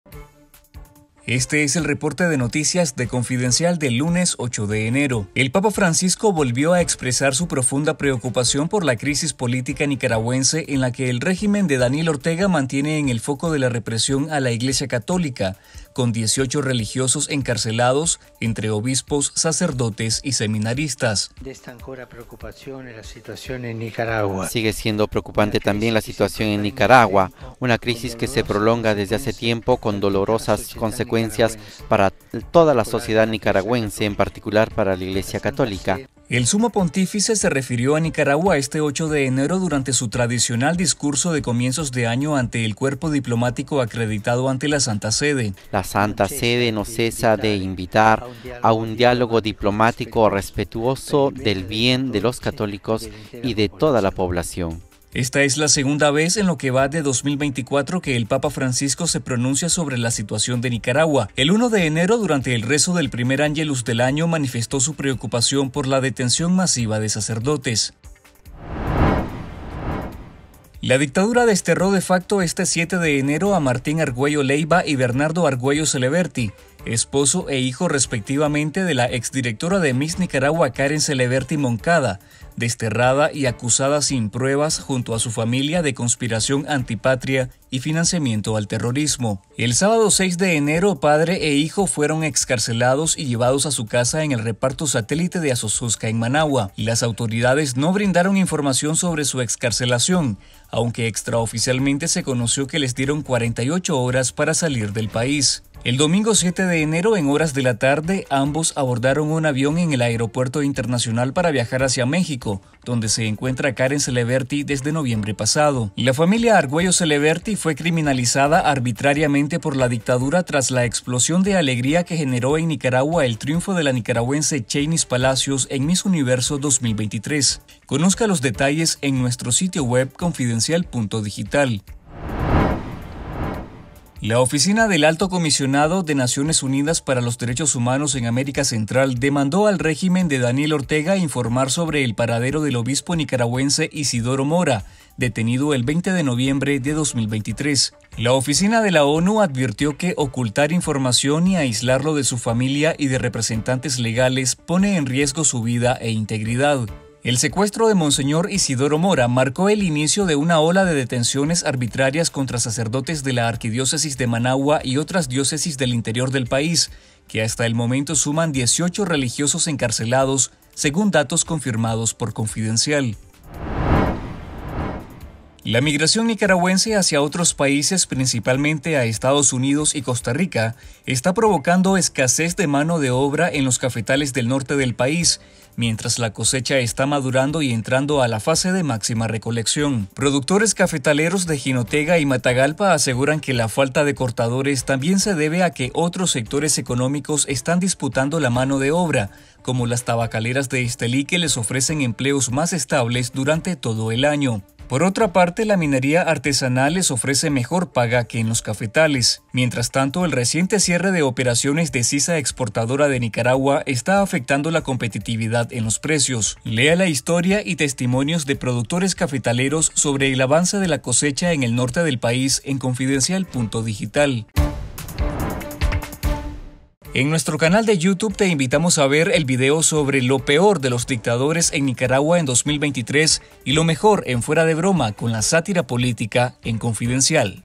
Papa Francisco se pronuncia sobre Nicaragua por segunda vez en 2024
El papa Francisco volvió a expresar su "profunda preocupación" por la crisis política nicaragüense, en la que el régimen de Daniel Ortega mantiene en el foco de la represión a la Iglesia católica con 18 religiosos encarcelados, entre obispos, sacerdotes y seminaristas.
Esta es la segunda vez, en lo que va de 2024, que el papa Francisco se pronuncia sobre la situación de Nicaragua. El 1 de enero durante el rezo del primer Ángelus del año, manifestó  su “preocupación” por la detención masiva de sacerdotes.

En otras noticias de nuestro reporte:
- Destierran a esposo e hijo de exdirectora de Miss Nicaragua, Karen Celebertti.
- ONU demanda información sobre el paradero del obispo Isidoro Mora.
- Faltan cortadores, mientras avanza maduración del café en Matagalpa y Jinotega.